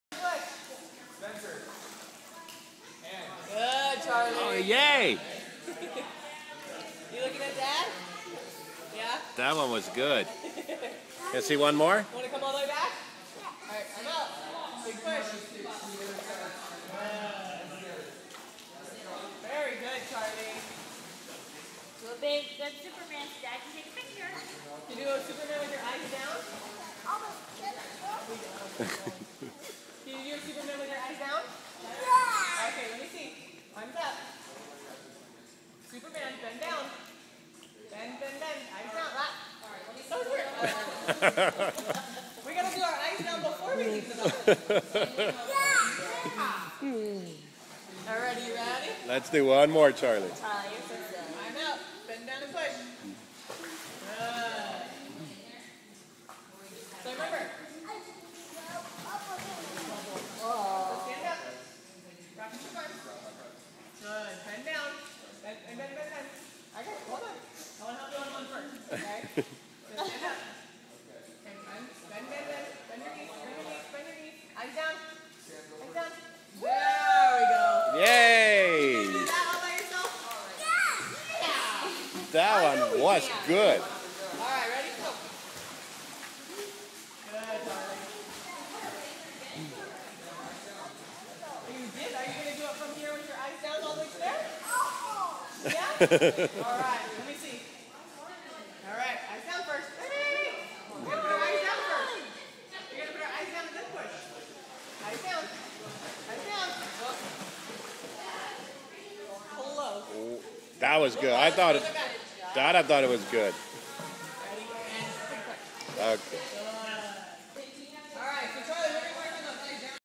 Charlie at Swim Class in Linda Vista
Charlie has been going to swim class for a while now and has made massive improvements over the last few months - including swimming under water! His teacher Miss Lisa (and teacher in training, Miss Jenny) has been great support.